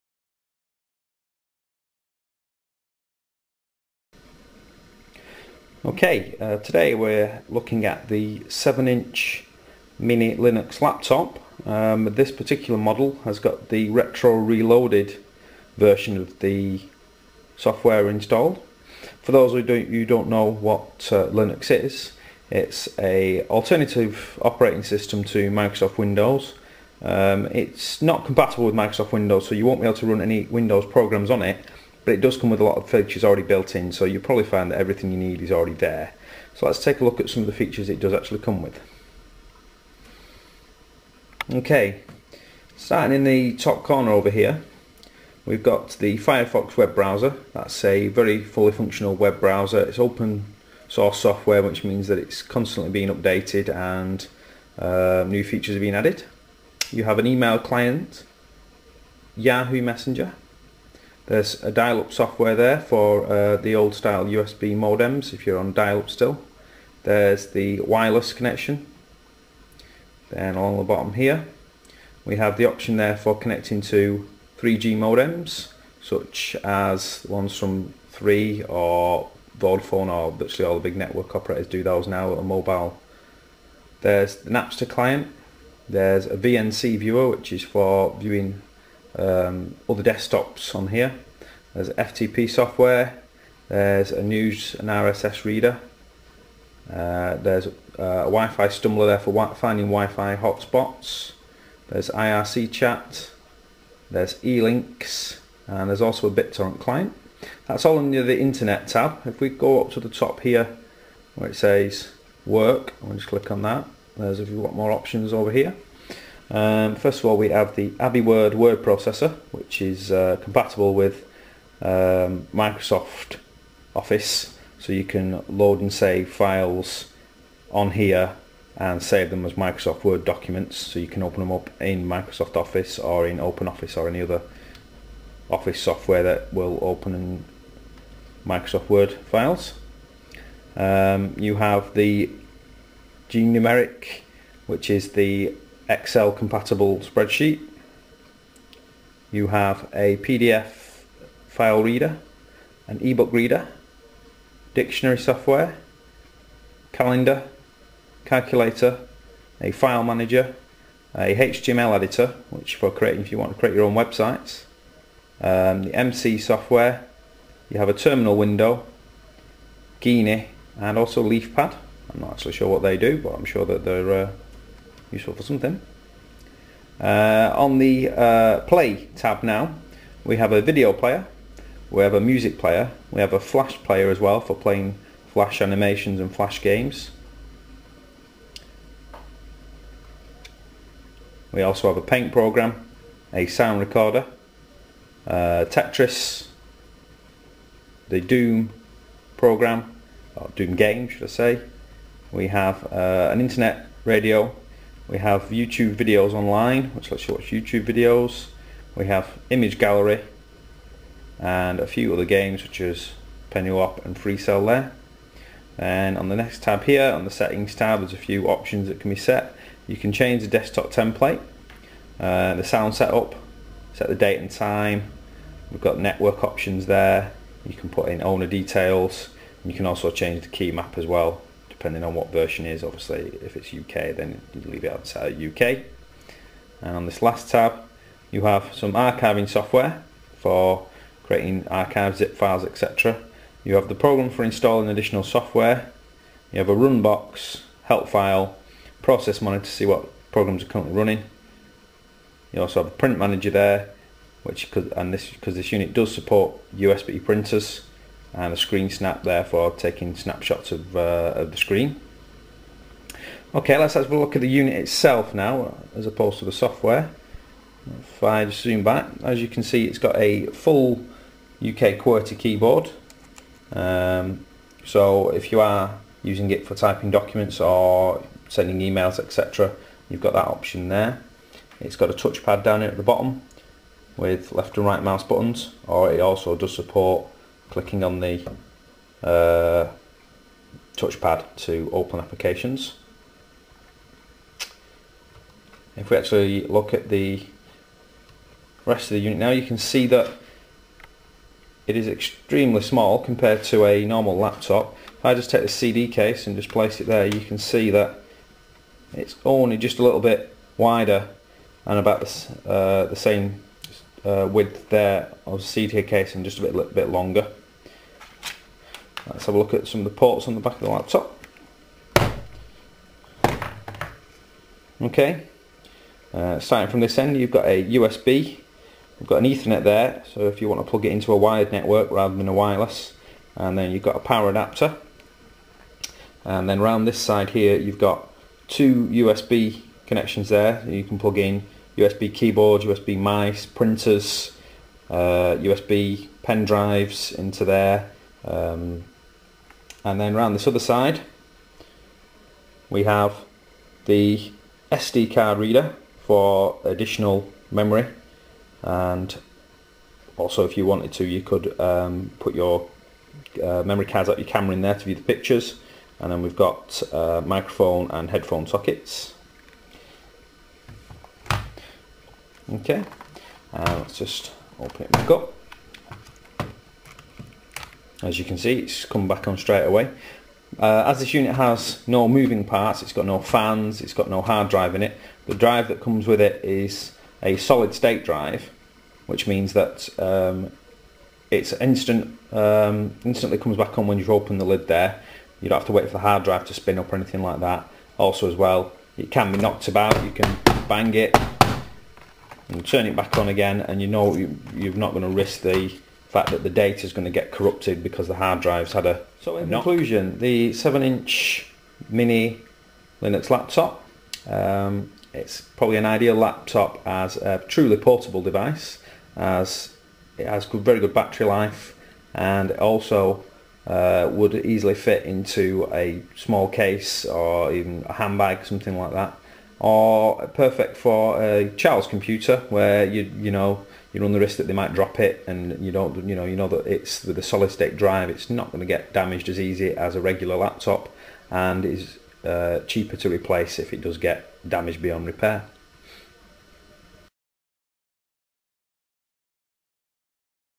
Cheap mini laptop running linux
A 7 inch mini laptop running Linux. integrated WIFI, 32bit CPU, 2GB flash drive, SD card reader.